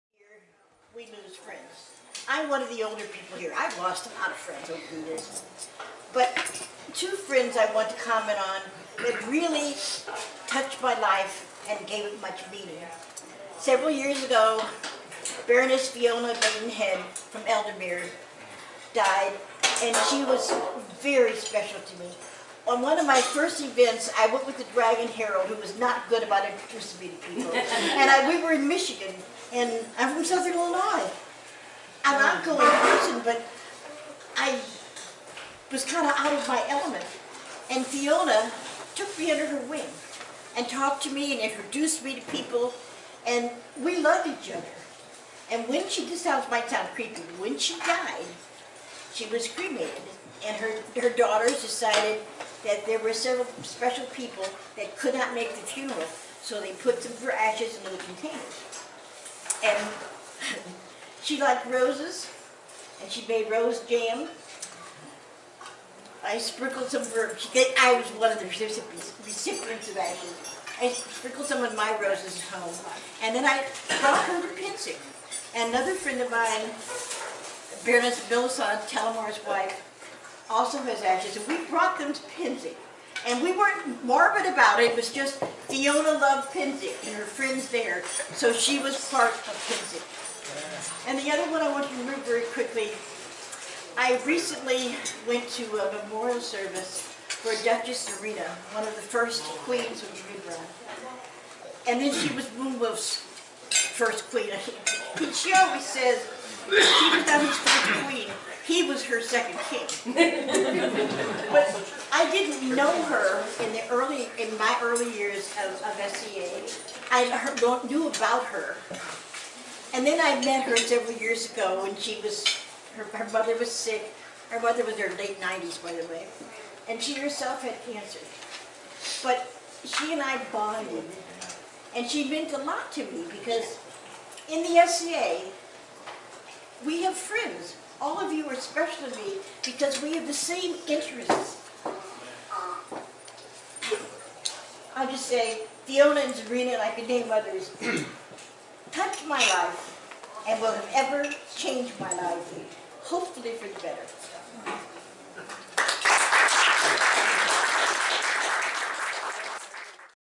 Caroline de Mercier: In Praise of Absent Friends
Caroline was up for my challenge. We often have challenges which need to be prepared "on the day", but normally we at least warn you of the nature of the challenge. This one is a total mystery, even to the Provost. I was the Patron and arrrived at the event and announced  ... well, the terms of this challenge when ... let's say, when it suits her.

Recorded at Midrealm’s Bardic Madness XVI, in the Shire of Ravenslake, 15 November, 2014.